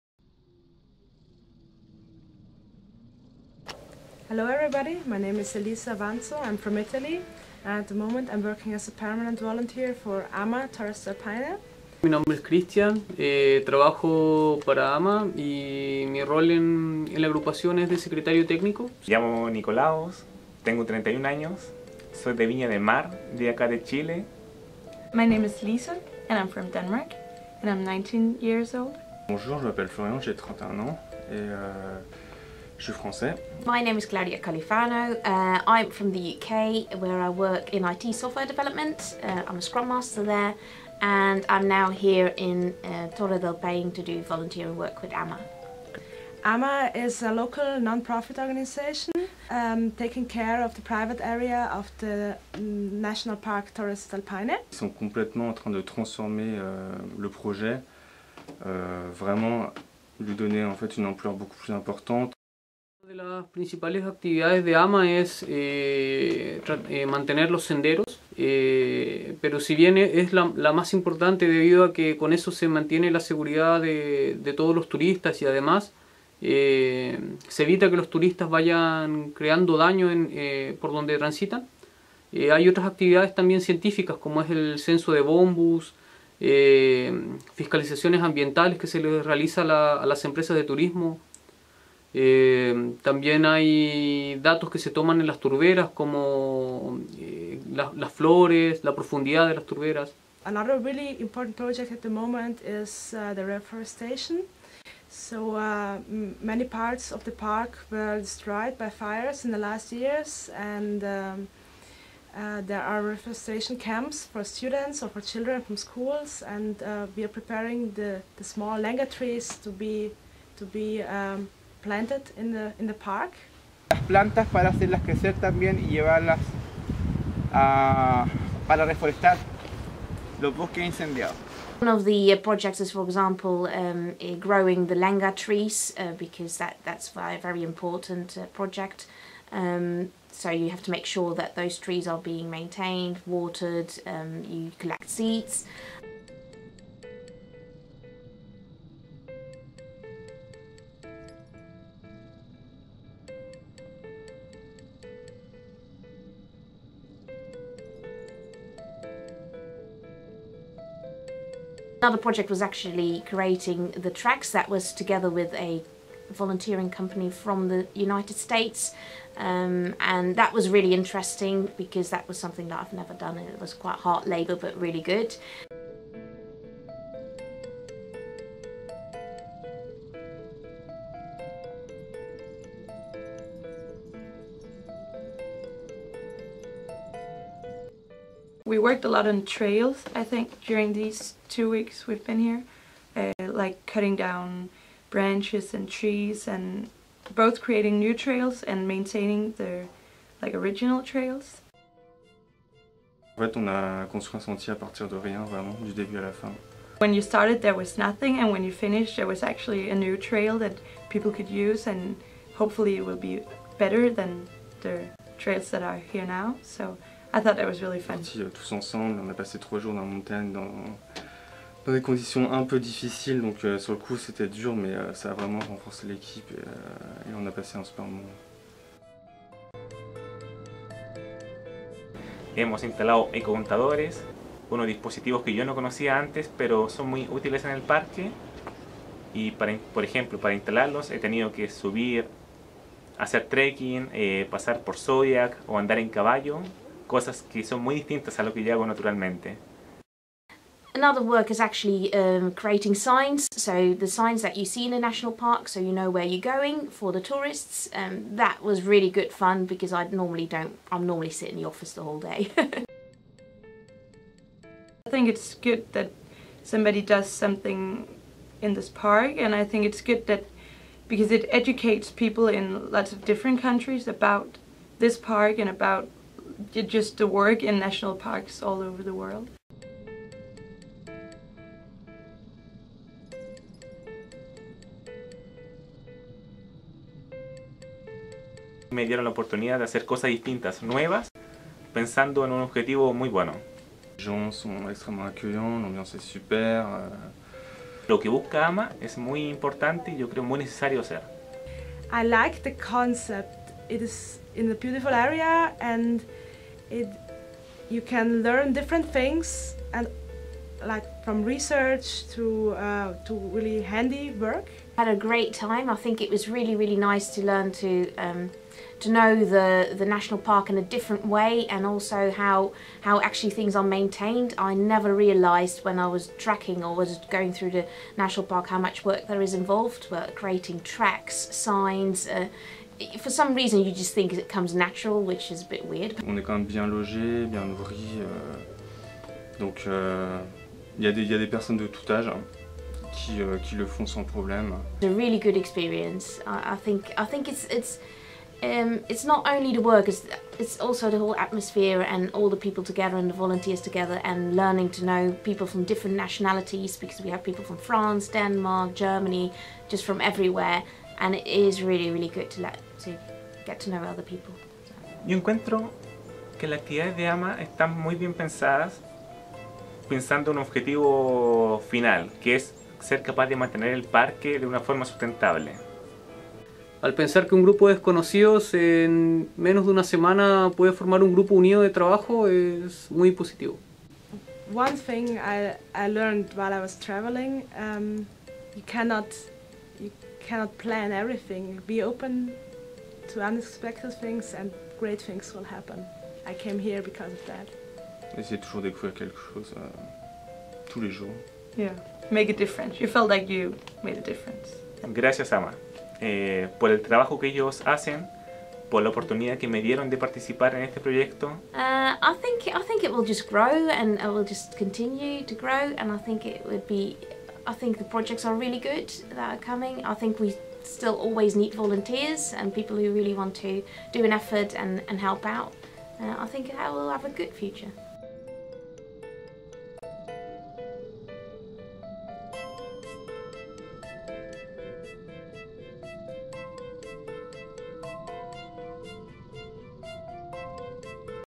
AMA Torres del Paine - Interview, avis et retour des volontaires - Reportage EVI
Rencontre avec l'association chilienne AMA Torres del Paine.
Interview des volontaires et des actifs.